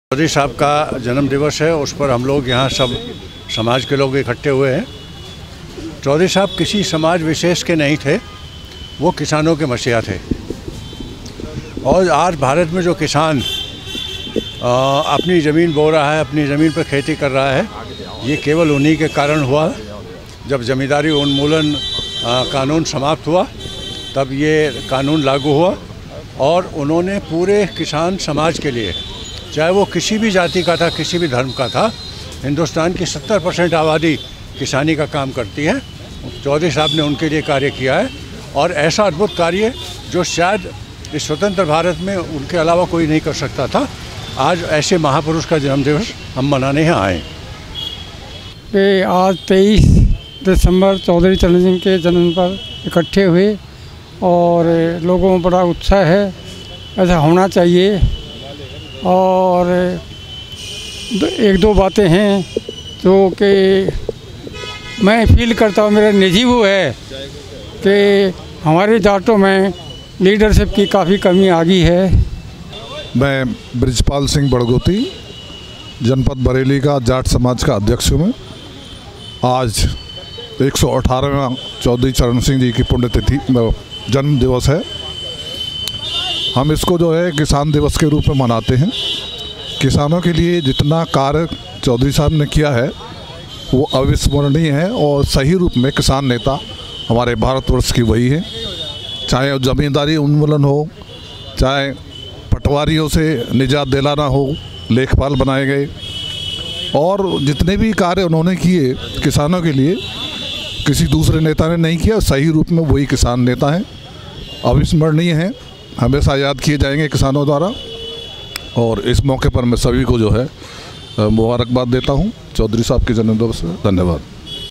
राष्रीय लोकदल ने मनाया पांचवें प्रधानमंत्री चौधरी चरण सिंह का जन्मदिन Bareilly
किसान दिवस पर क्या बोले पूर्व मेयर आई. एस तोमर ?